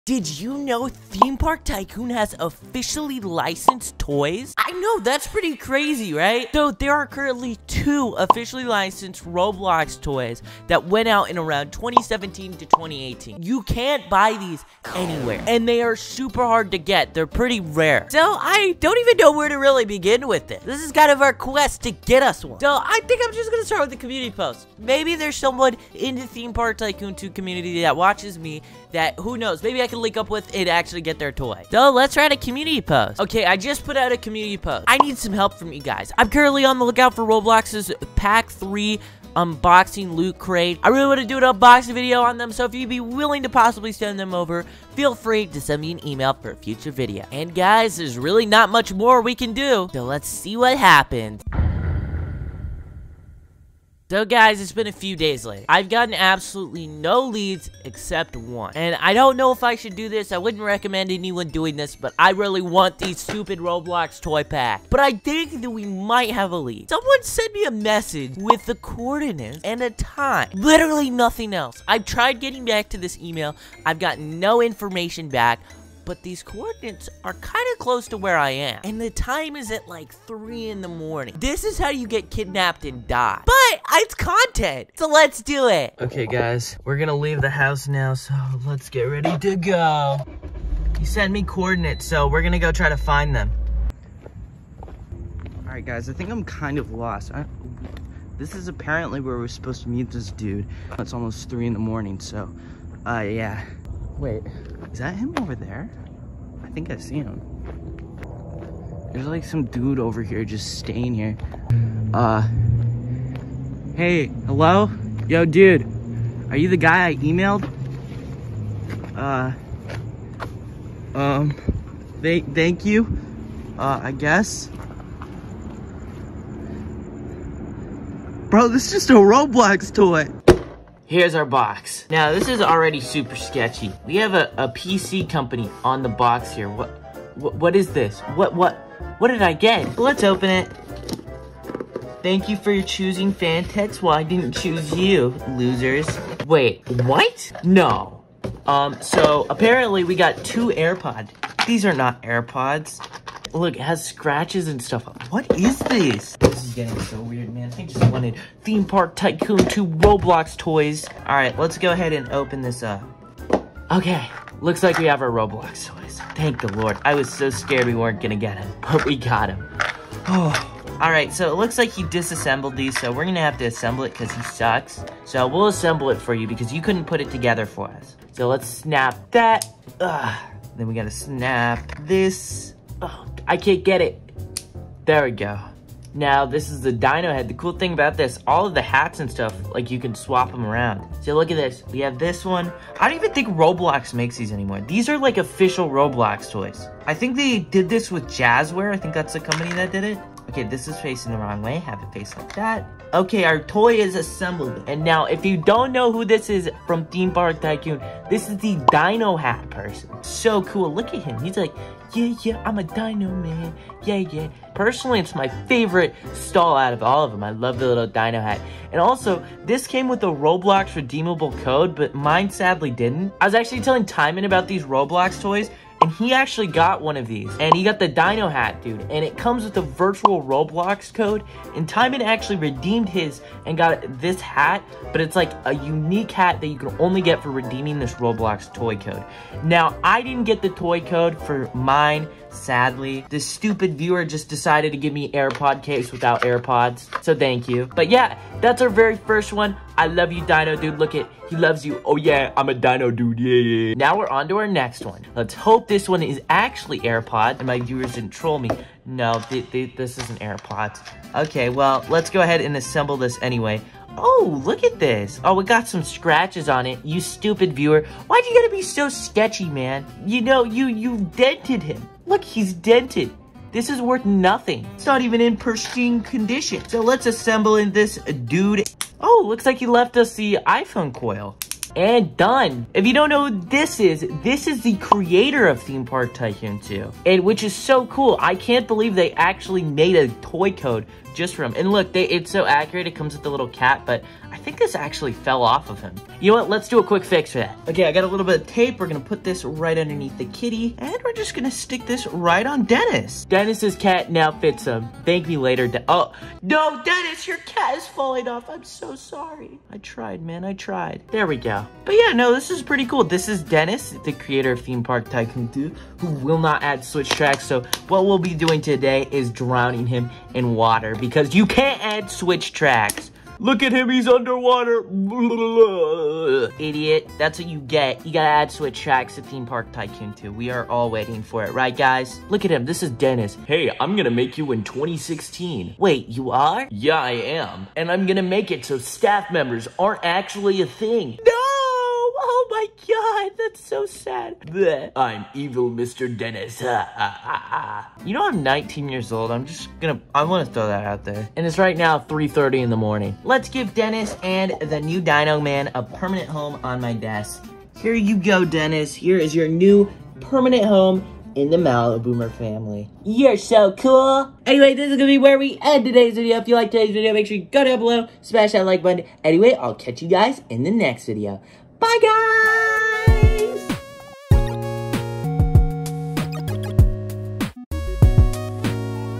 Theme Park Tycoon 2 *TOY* Unboxing! (RARE TOYS) 📦😲
Today we UNBOX Official theme park tycoon 2 TOYS! Yes These are REAL officially licensed Roblox Toys of Theme Park Tycoon 2! We unbox the den_s jazware roblox toys based on theme park tycoon 2! We build 2 RARE Roblox toys that are NO LONGER SOLD. These Theme park tycoon 2 toys ARE SO COOL! I hope den_s makes a contract and gets more of these theme park tycoon 2 toys!

(btw i didnt actually meet anyone it was fake if u rlly couldn't tell LOL, don't do this XD)

Like & Sub!! :DDD 💖👍

→Outro Credits:
Transition: Kosii
Song: gijs.i